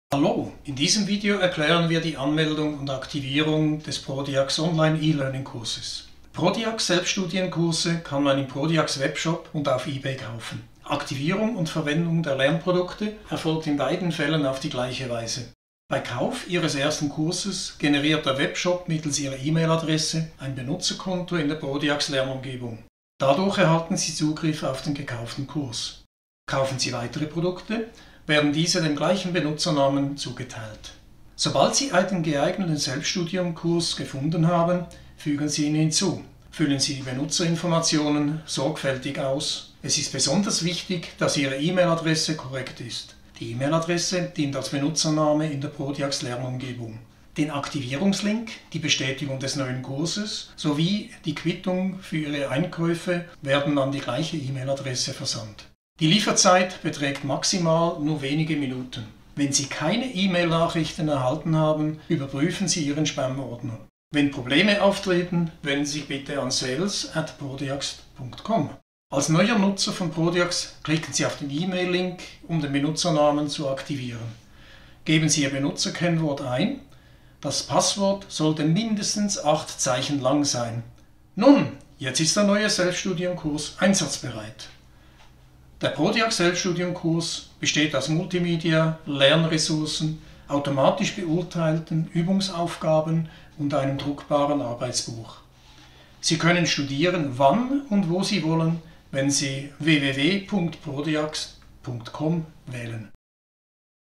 Wie beginne ich mit dem Prodiags Selbststudium Online Automobiltechnik Lernkurs?
In diesem Video erklären wir die Anmeldung und Aktivierung des Prodiags online e-Learning Kurses